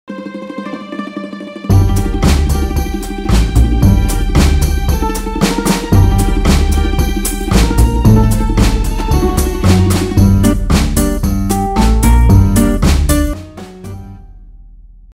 Dia do Circo
Vinheta produzida pela TV UFG para o Dia Mundial de Combate à Tuberculose / Concepção: Todos os conteúdos realizados pela Fundação RTVE / TV UFG são produzidos de forma colaborativa / Produção: Vasconcelos Neto / Roteiro: Adérito Schneider / Imagens cedidas por: Vasconcelos Neto / Edição e videografismo: Alex Sapiência